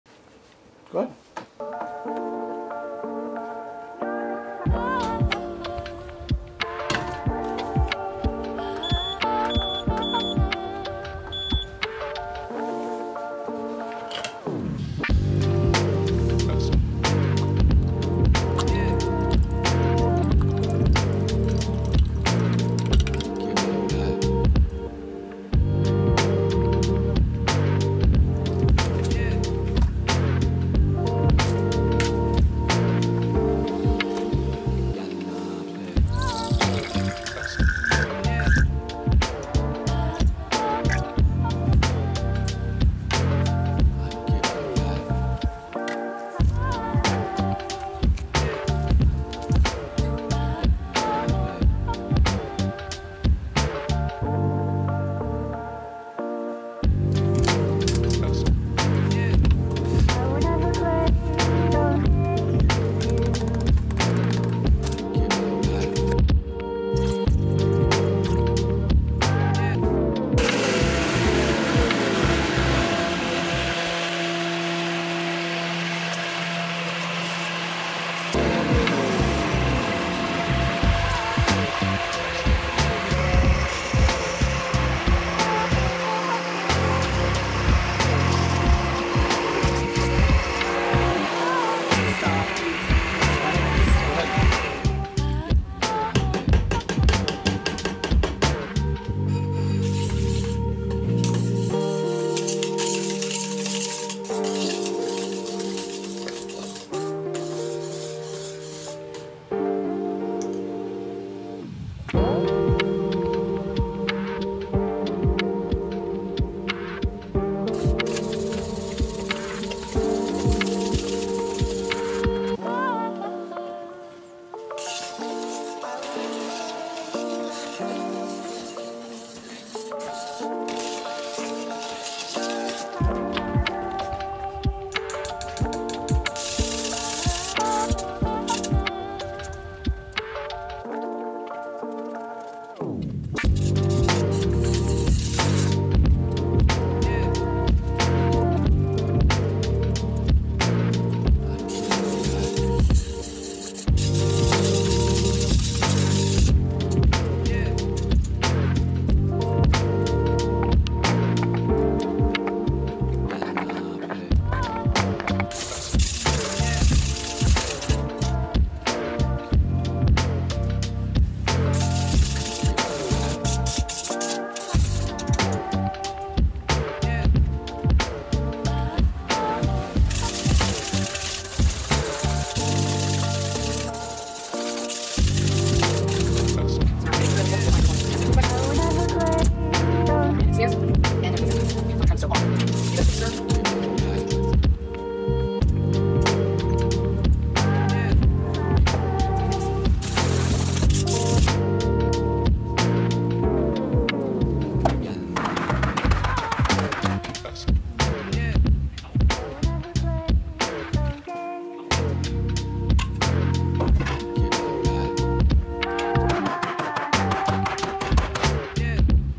Making Shampoo and Body Bars: Start to Finish
In this video we show you how we handmake our shampoo and body bars from start to finish. These are made with extra virgin olive oil, lye and essential oils only. After they are made they cure for about two days we remove them from the mold and cut them.